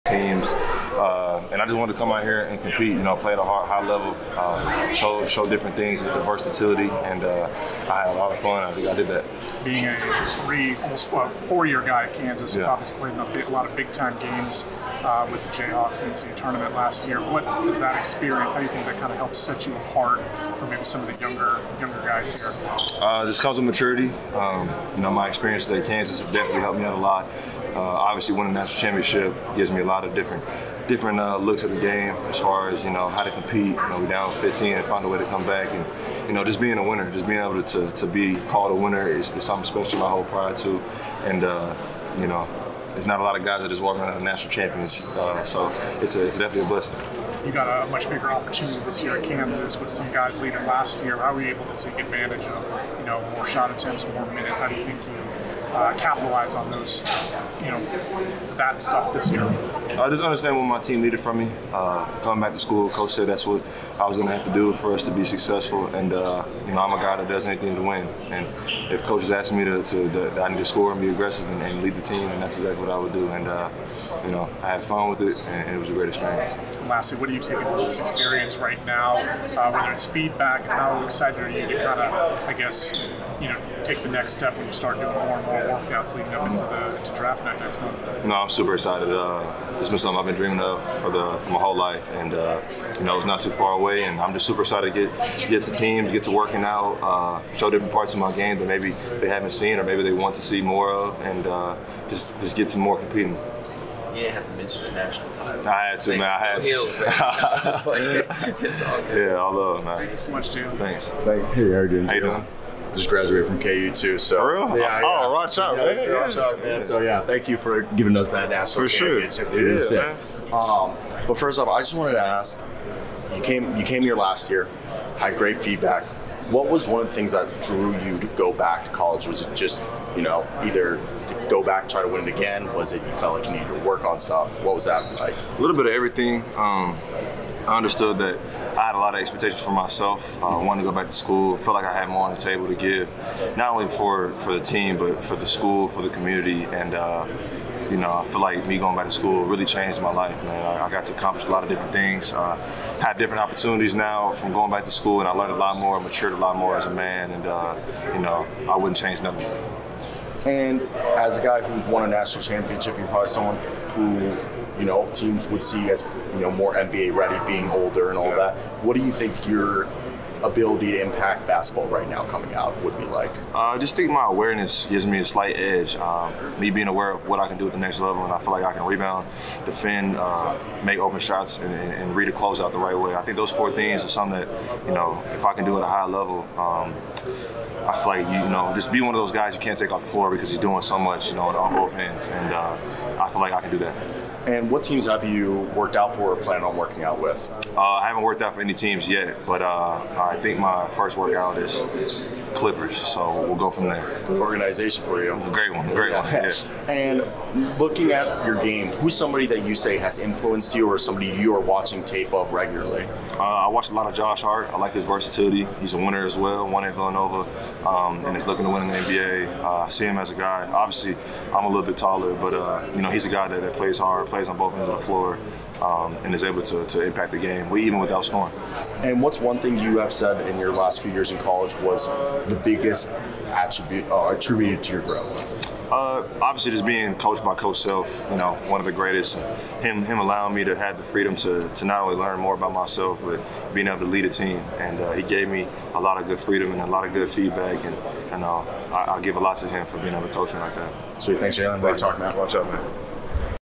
Jalen Wilson 2023 NBA Draft Combine Interview
This interview is from the 2023 NBA Draft Combine in Chicago, Illinois held between May 16th and 19th, serves as an opportunity for prospective basketball players to showcase their skills and abilities to NBA teams ahead of the upcoming draft. During the combine, players participate in a series of physical tests, measurements, and basketball drills, allowing teams to assess their athleticism, shooting, passing, and overall basketball prowess. This interview session provides media personnel with a chance to engage with the talented individuals participating in the combine. It offers a platform for journalists, reporters, and broadcasters to ask questions and gain insights into the prospects' mindset, aspirations, and preparation for the draft. The interviews aim to shed light on the players' backgrounds, strengths, weaknesses, and potential contributions to the NBA.